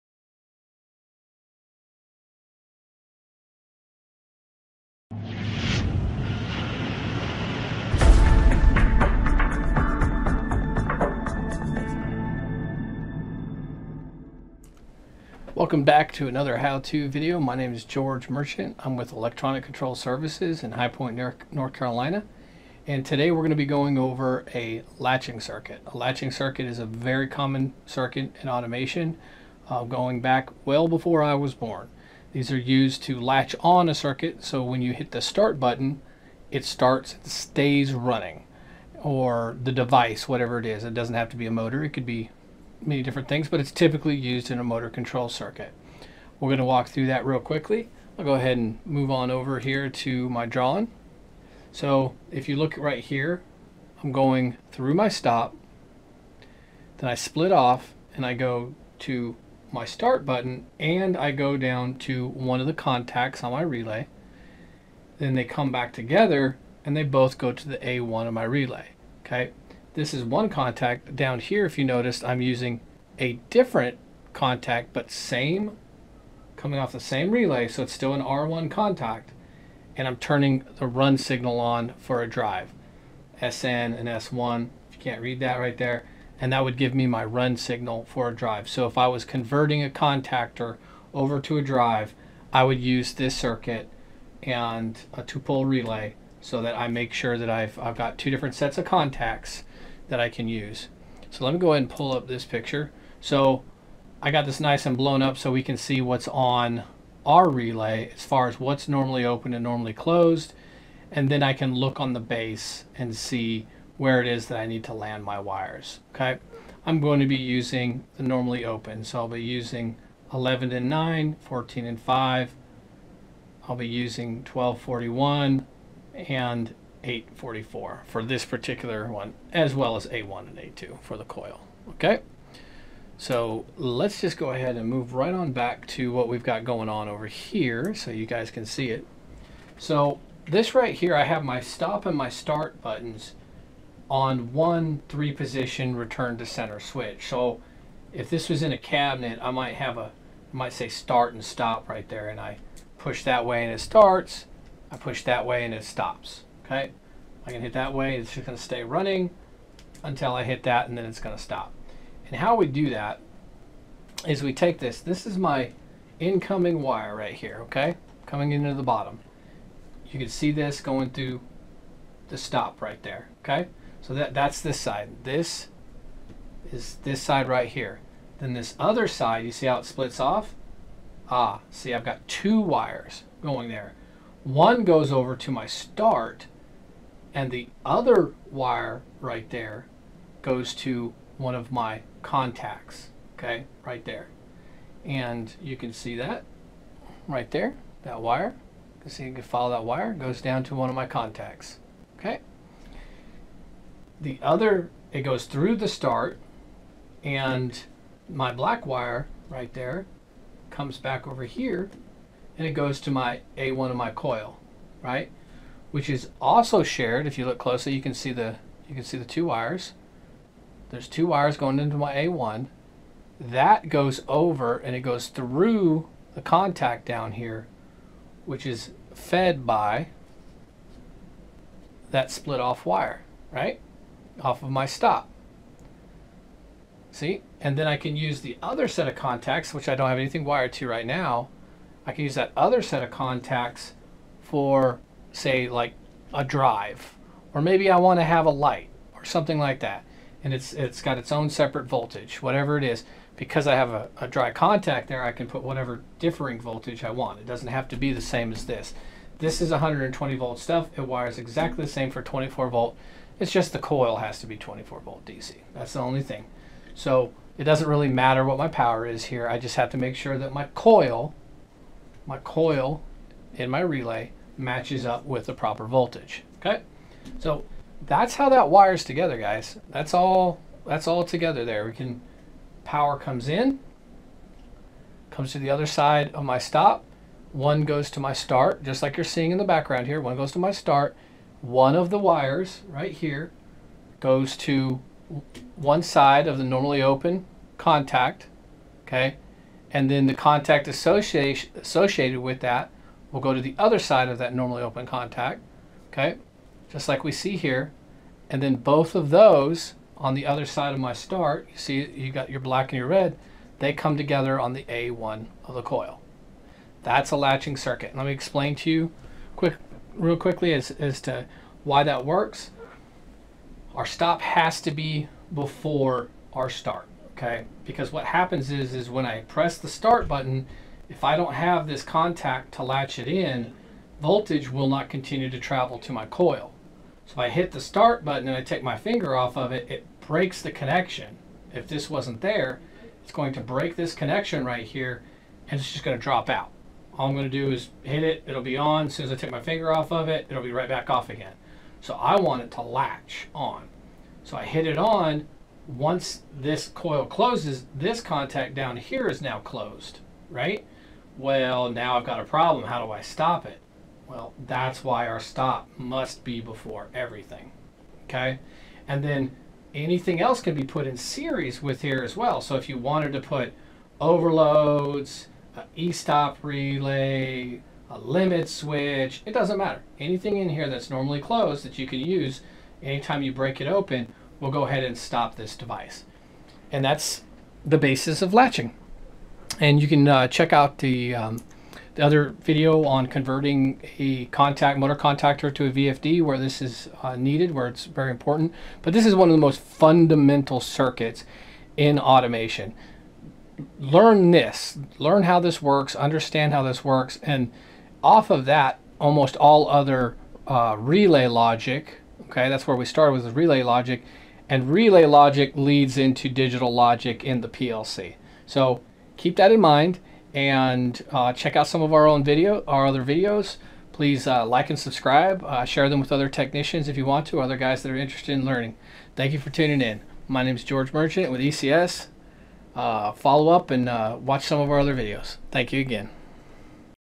How to wire up a latching circuit
How to wire up a latching circuit used in Industrial controls.